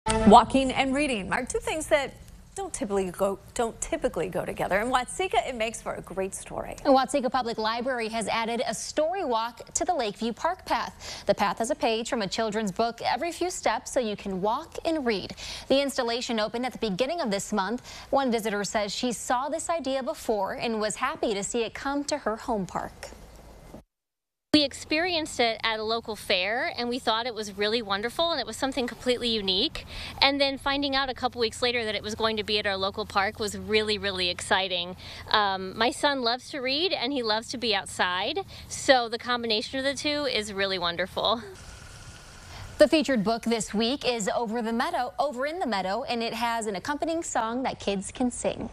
Watseka Public Library combines walking and reading at park path
Walking and reading are two things that don't typically go together, but in Watseka, it makes for a great story. The public library has added a story walk to the Lakeview Park walking path.